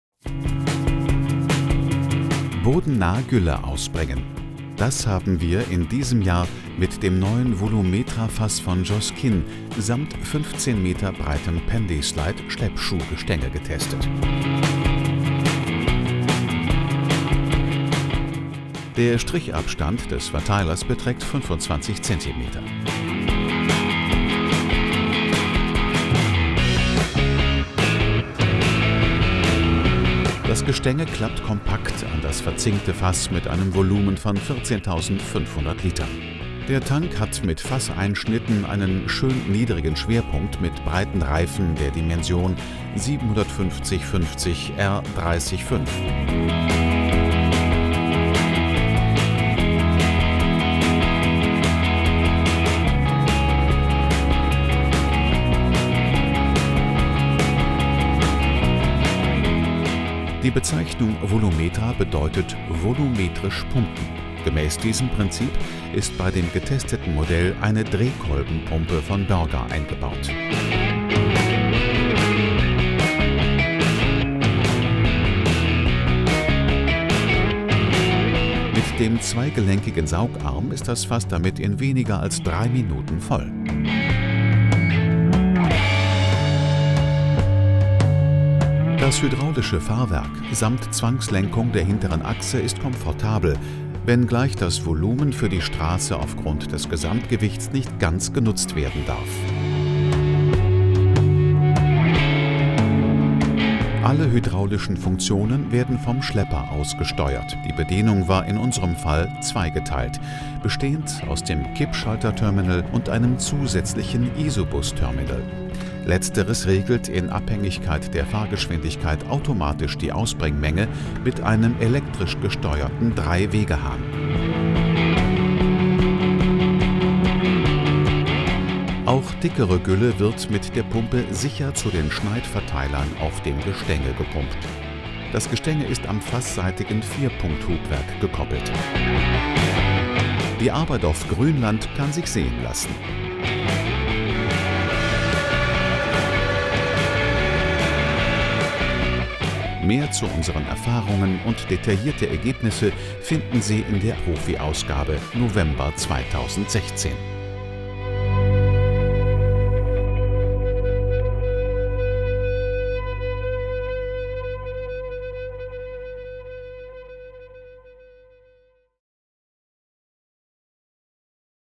Joskin Volumetra 14 500 D
Komplizierte Leistung: Joskin hat im vergangenen Jahr die neue Volumetra-Güllefassbaureihe vorgestellt, die mit Exzenterschnecken- oder Verdrängerpumpen ausgerüstet ist. Nach 1 500 m³ ausgebrachter Gülle lesen Sie hier unser Urteil.

Ach ja, die Preise;-)
Grundausstattung:   51980 Euro
Testausstattung:    100753 Euro
zzgl. Gestänge:         30460 Euro

Credits:
Autor: Tobias Bensing
Kamera: Tobias Bensing
Schnitt: Roman Hünefeld
Sprecher: Christoph Wittelsbürger
Postproduktion: Roman Hünefeld